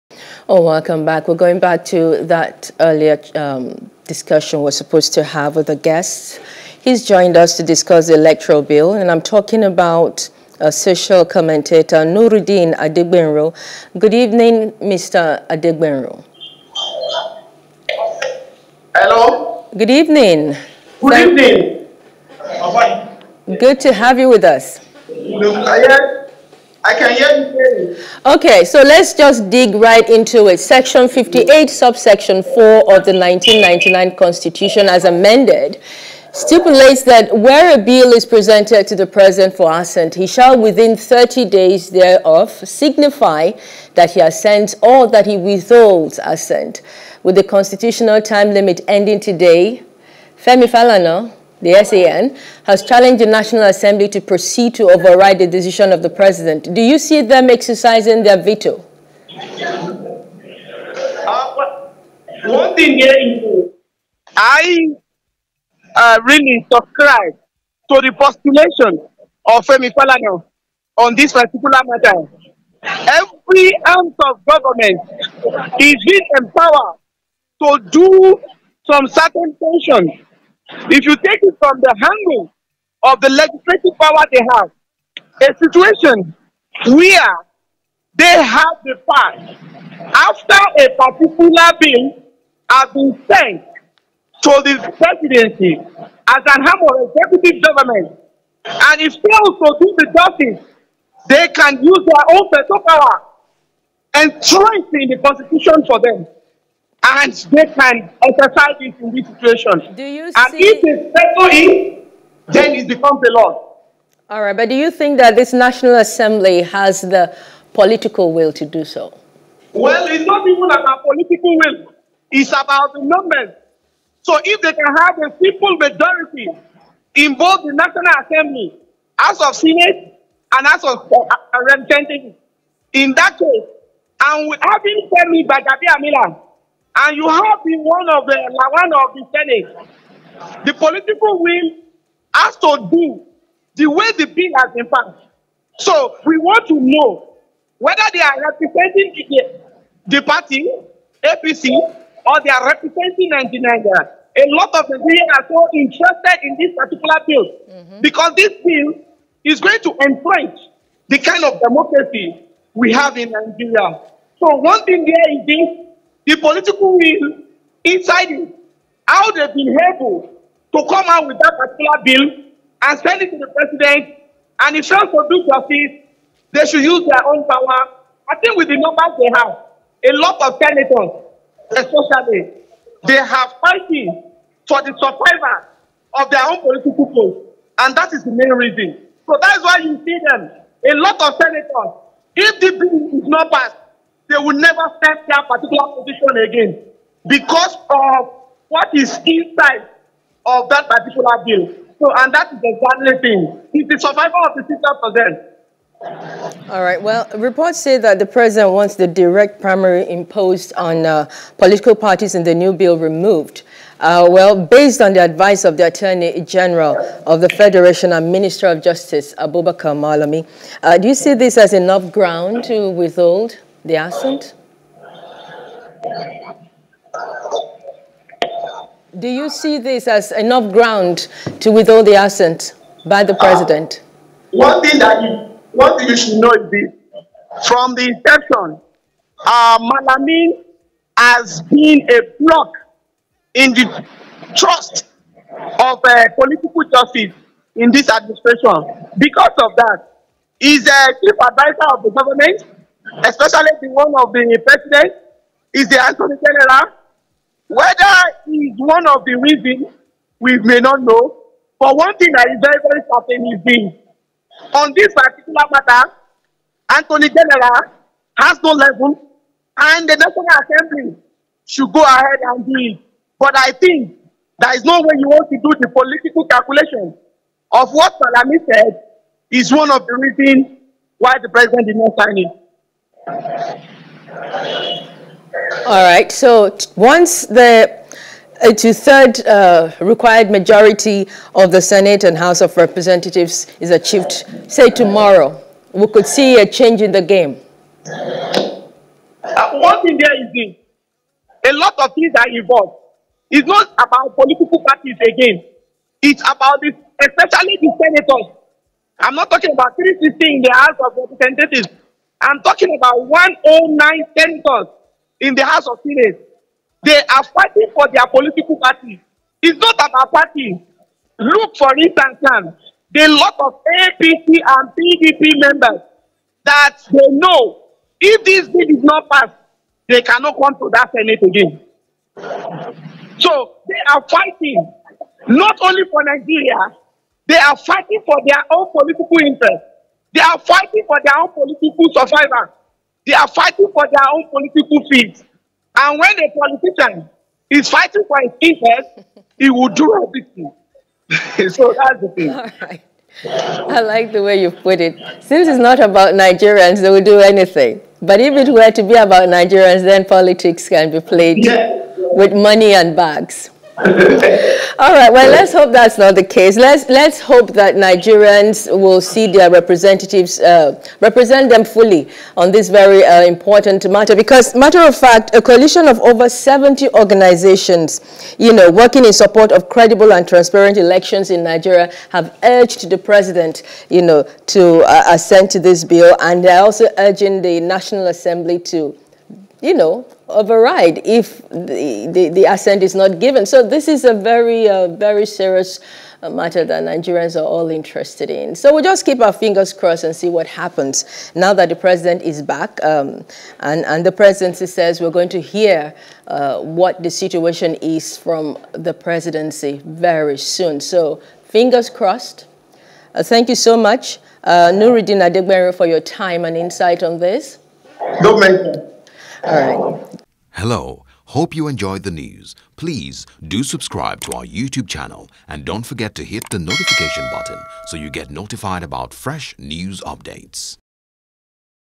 Electoral Bill: Buhari Returns From Turkey As Deadline Ends | NEWS
President Muhammadu Buhari has returned from Turkey after attending the three-day Turkey-Africa Partnership Summit in Istanbul.

Social commentator, Nurudeen Adegbenro joined us to discuss.